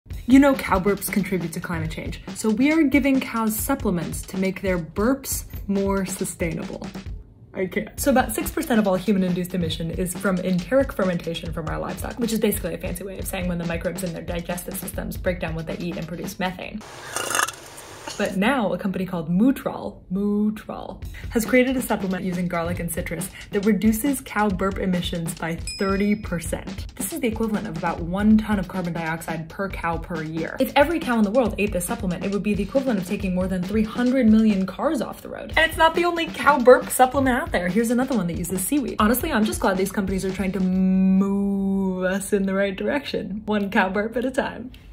Making cow burps... more sustainable?
Cow burps contribute to climate change, so we’re giving them supplements to make their burps… more… sustainable.

 *cow burp*

Yeah.